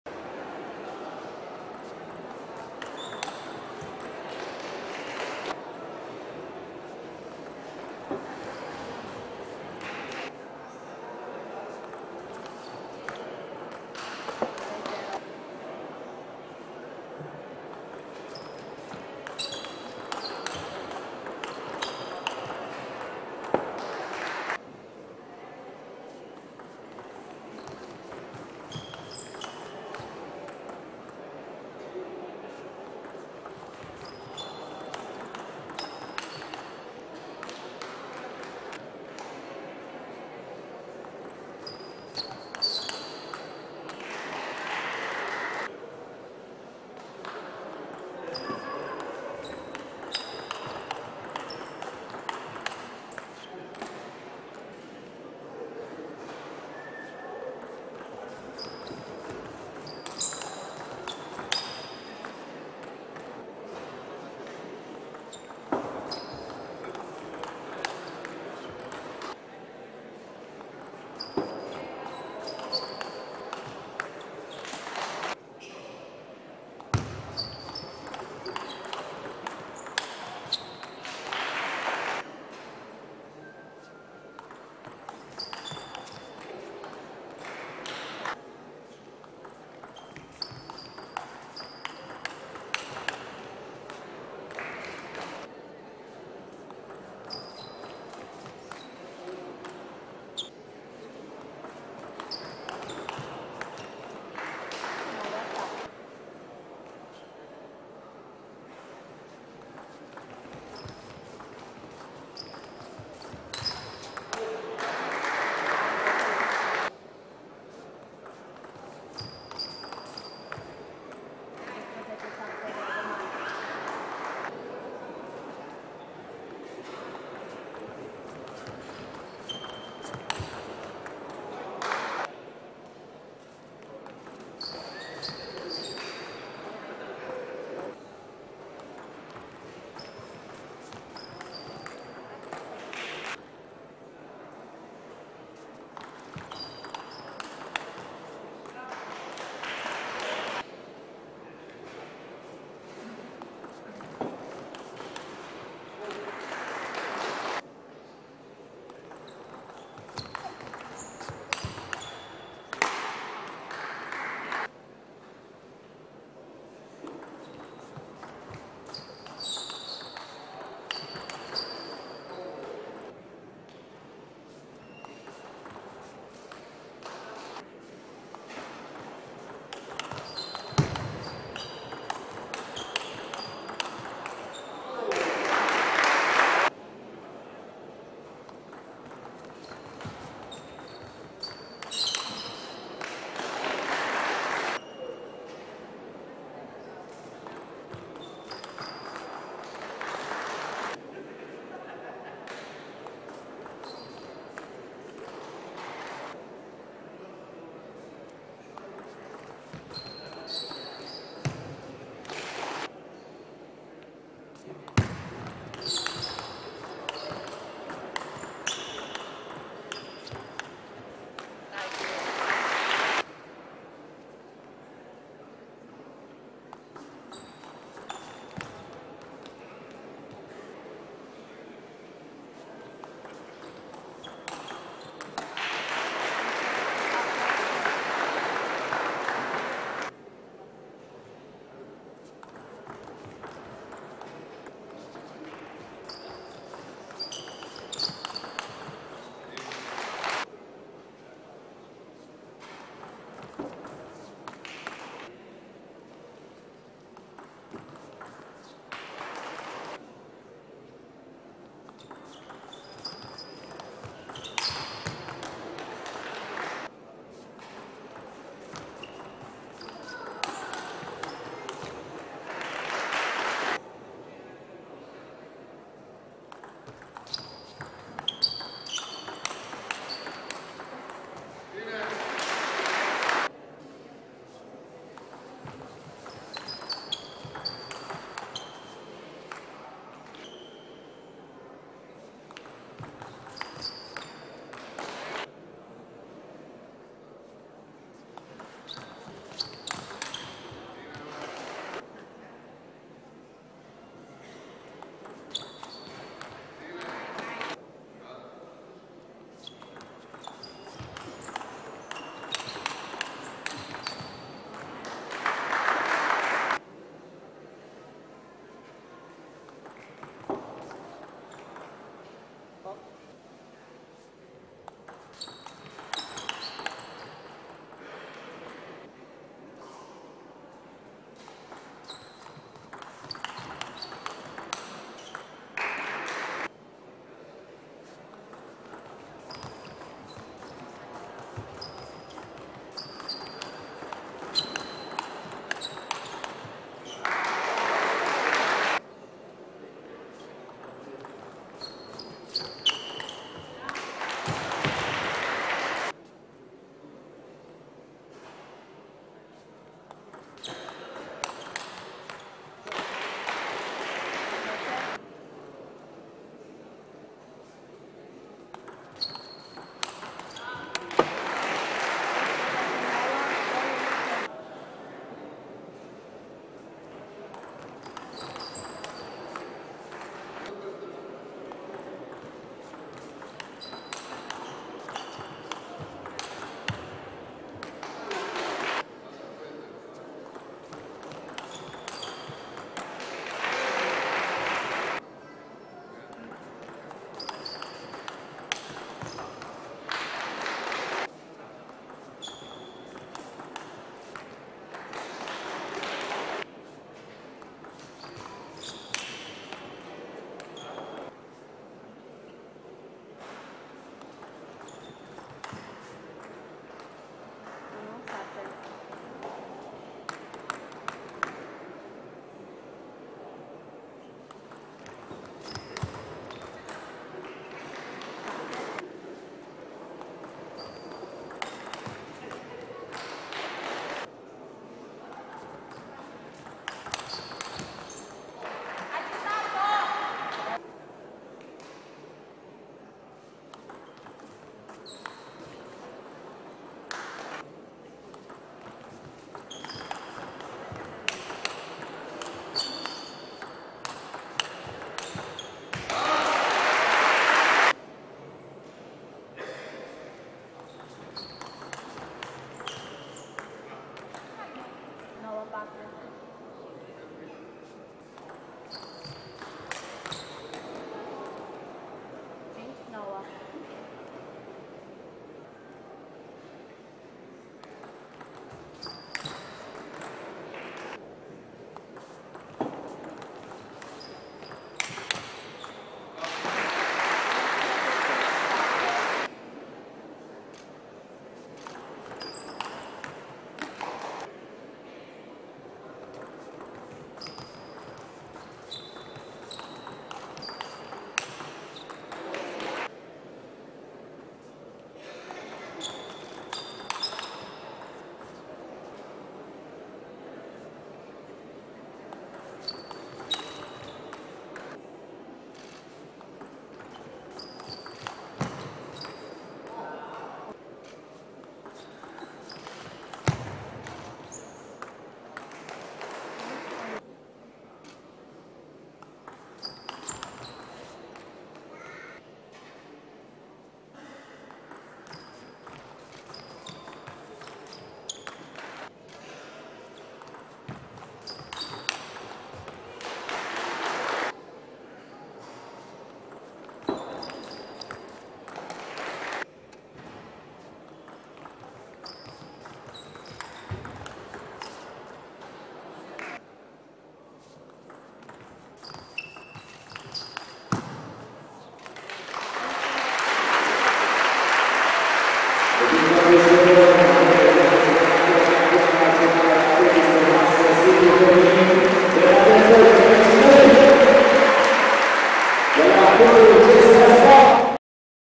Bernadette Szocs vs Andreea Dragoman | FINAL | 2023 Romania National Championships
©FRTM @frtm-romaniantabletennisfe5257